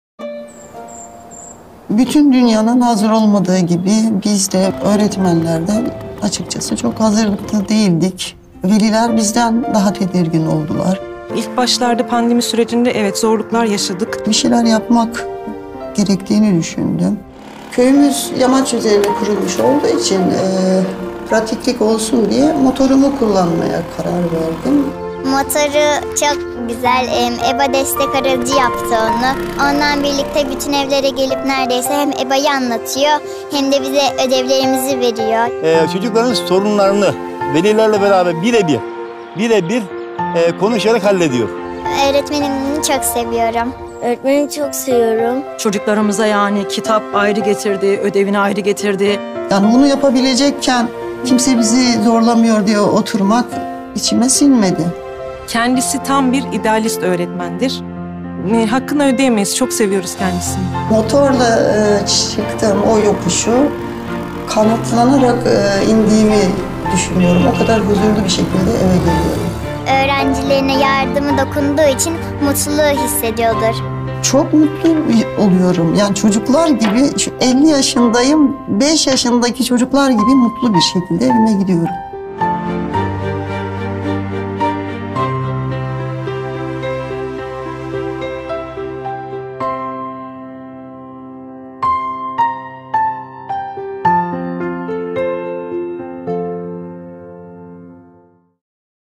Geyve ilçesindeki Nuruosmaniye köy okulunda EBA destek aracı Emine Erdoğan Twitter'dan paylaştı.
Emine Erdoğan, Sakarya'nın Geyve ilçesindeki Nuruosmaniye köy okulunda görev yapan ve motosikletini EBA destek aracına dönüştürerek uzaktan eğitime destek veren öğretmen Nuray Yıldırıcı'nın videosunu Twitter'dan paylaştı.

İçinden geçtiğimiz zorlu pandemi şartlarında öğretmenlerimiz, çocuklarımızın okulla bağının kesintiye uğramaması için ter döküyorlar. Kapı kapı dolaşıp, çocuklara eğitim materyalleri dağıtan emektar tüm eğitimcilere şükran borçluyuz.